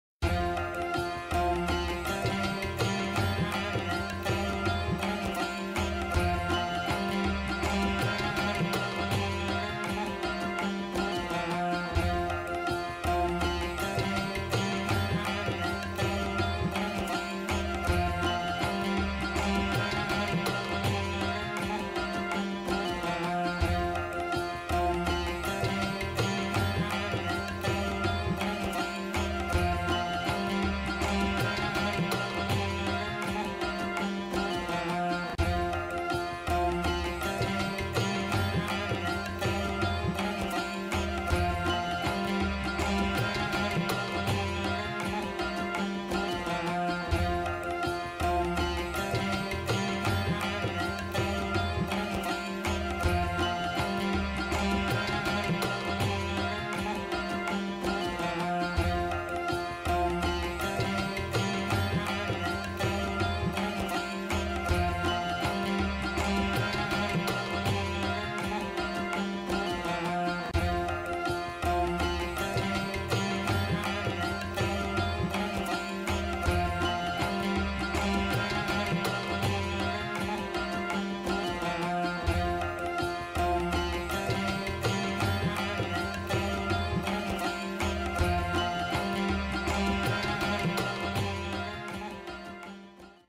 apartment kolam | குட்டி கோலம் | cute kolam | simple kolam designs | 7 pulli kolam | easy rangoli
It is small and easy for beginners to learn & draw kolam with 7 dots 3 times to 3 dots. 7x3x3 dots kolam.